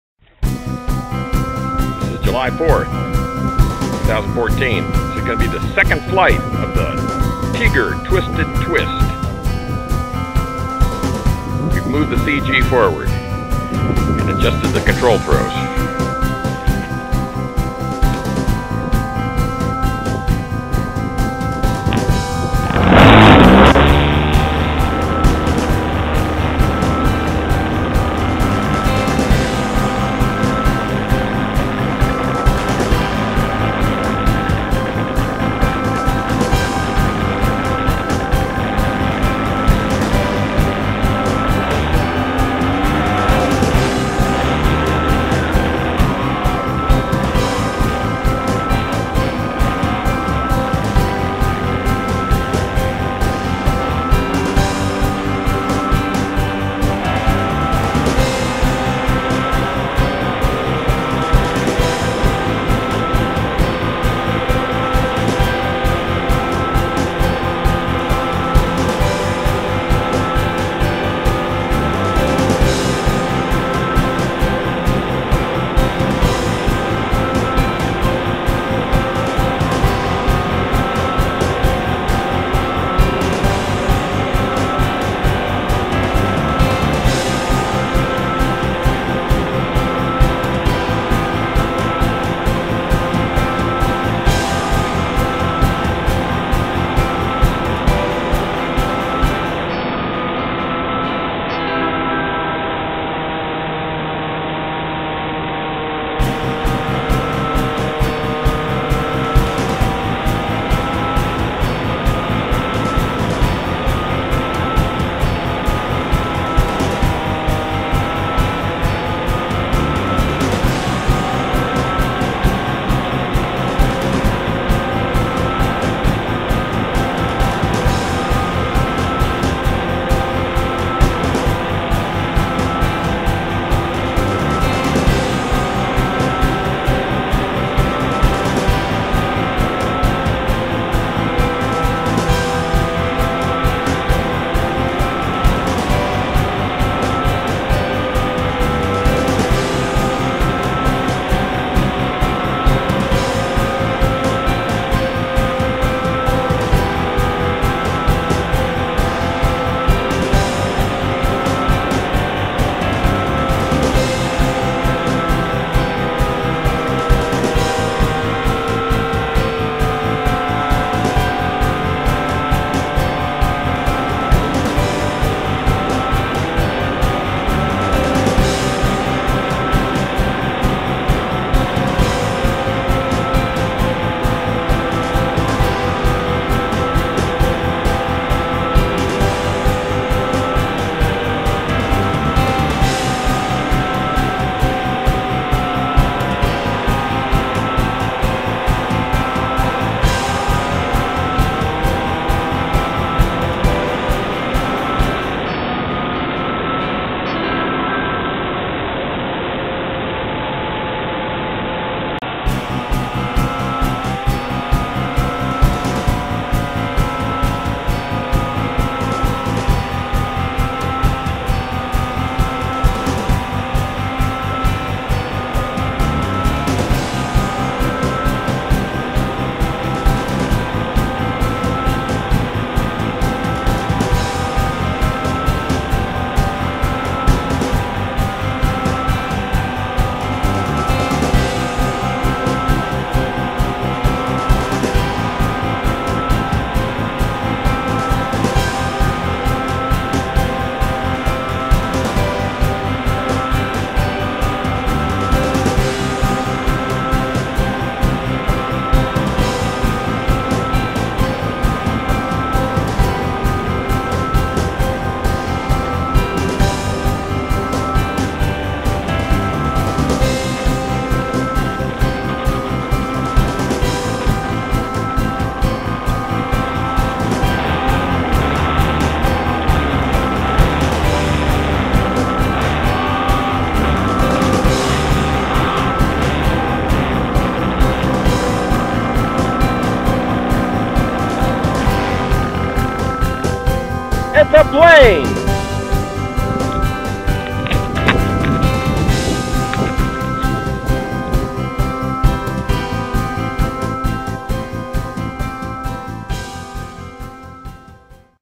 2nd Flight of Tigger Twisted Twist
Twist re-born after a fatal crash into the asphalt.  2nd flight after a harrowing maiden flight.